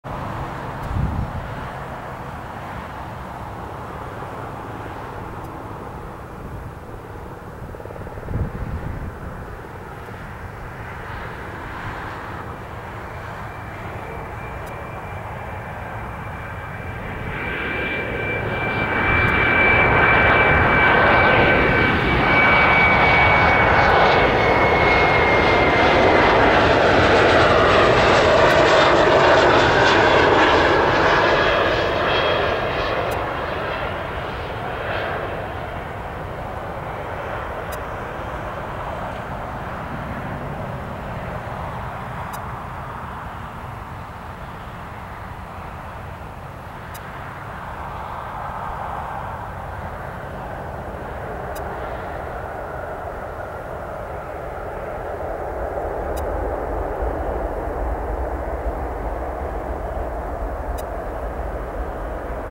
Alaska Airlines N703AS 737-400 Landing Portland Airport (PDX)
Flight: ASA659 PHX-PDX